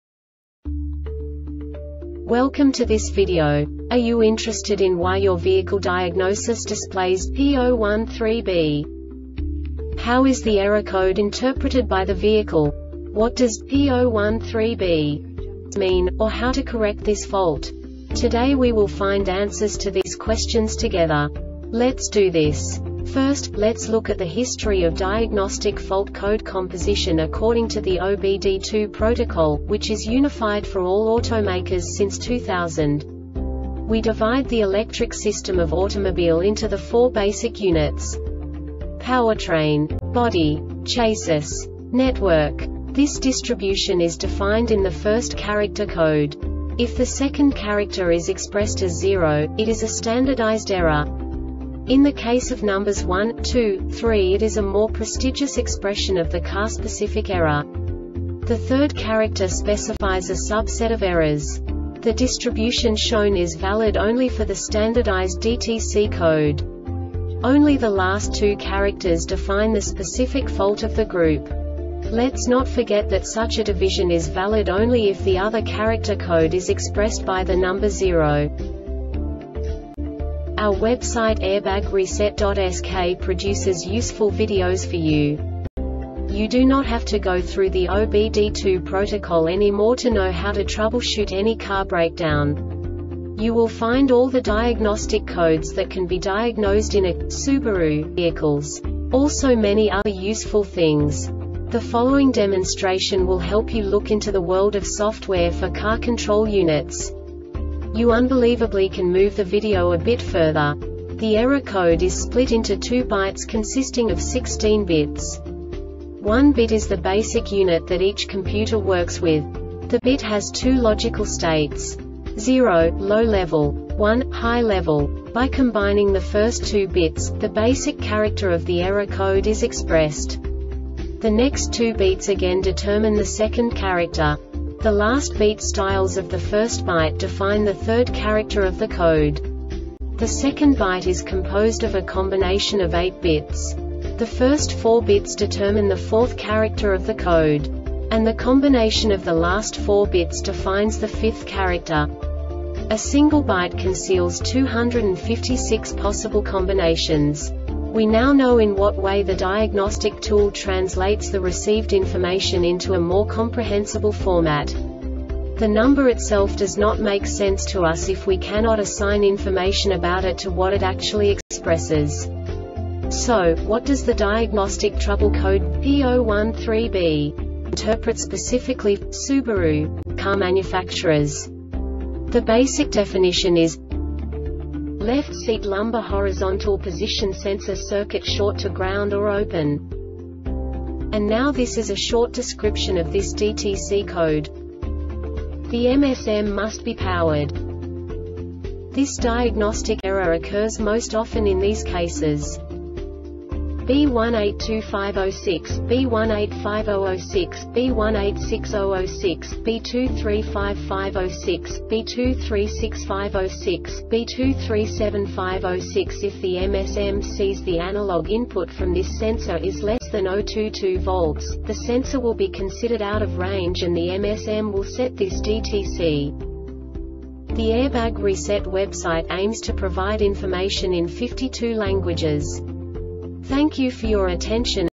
DTC Subaru P013B Short Explanation
Contents:
0:21 Basic DTC analysis according to OBD2 protocol standard.
1:48 Insight into programming
2:58 A diagnostically understandable description of the Diagnostic Trouble Code
3:05 Basic error definition

Make:
Subaru
Code:
P013B
Definition:
Left Seat Lumbar Horizontal Position Sensor Circuit Short to Ground or Open
Description:
The MSM must be powered.
Cause:
B1825 06, B1850 06, B1860 06, B2355 06, B2365 06, B2375 06If the MSM sees the analog input from this sensor is less than 0.22 volts,the sensor will be considered out of range and the MSM will set this DTC.